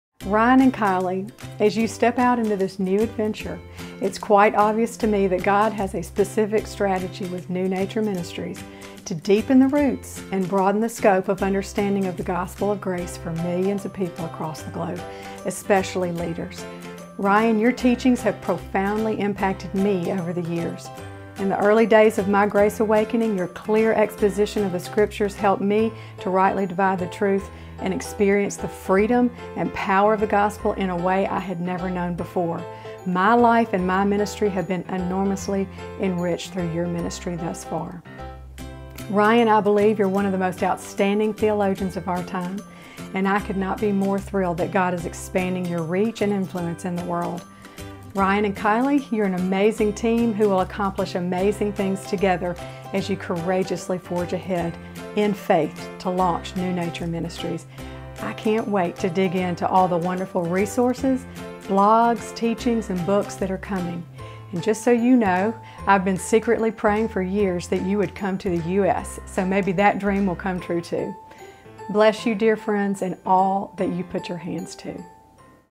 Tricia Gunn endorses New Nature Ministries (by Ryan and Kylie Rufus)
Tricia Gunn is a powerful Bible teacher who has a deep revelation of the Gospel of grace. She is active in preaching and developing teaching resources.
for lots of great content on the Gospel of Grace.